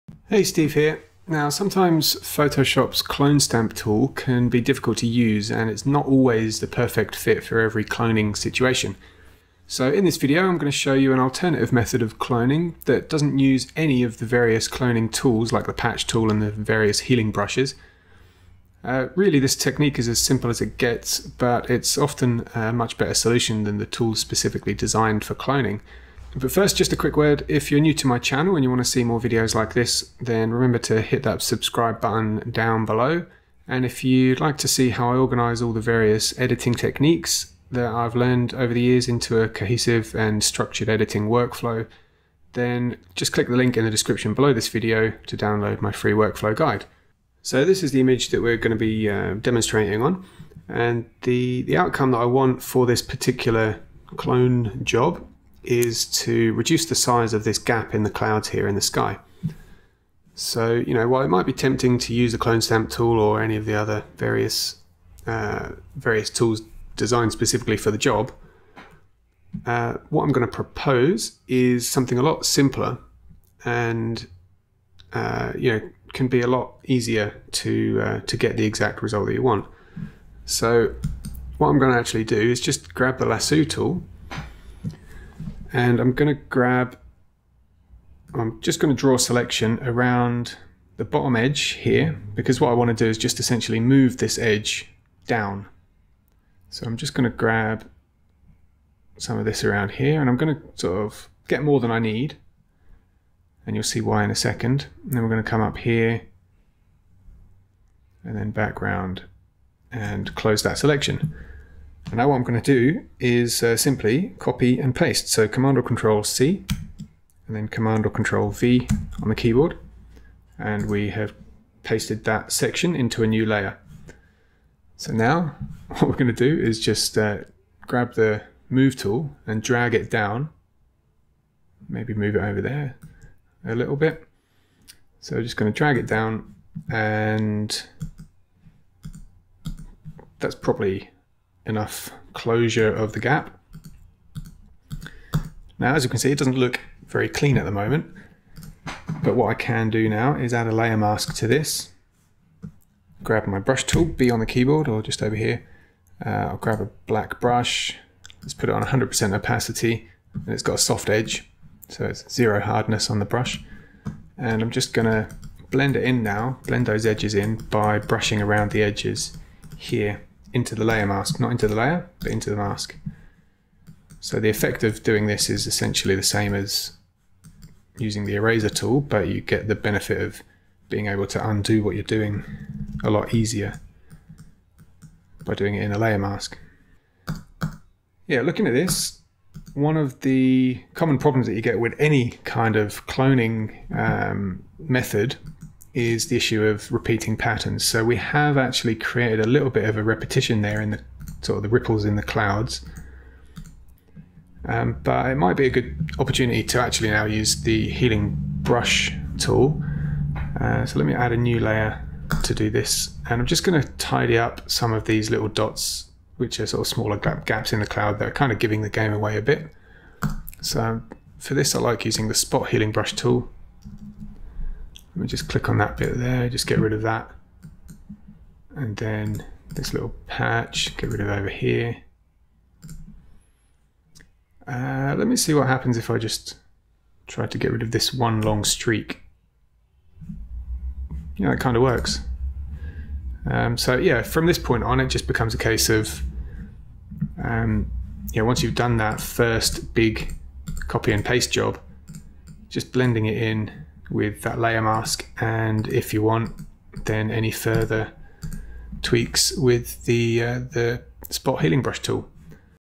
DUMB clone tool alternative
Sometimes Photoshops Clone Stamp Tool can be difficult to use, and it's not the perfect fit for every cloning situation.

In this video I'll show you an alternative method of cloning that doesn't use any of the various cloning tools (like the patch tool and the various healing brushes)...

This technique is as simple as it gets, but is often a better solution than the tools specifically designed for cloning...

And if you'd like to see how I organise all the seemingly random editing techniques I've learned and taught over the years into a cohesive and structured editing workflow, then go here to download my free workflow guide: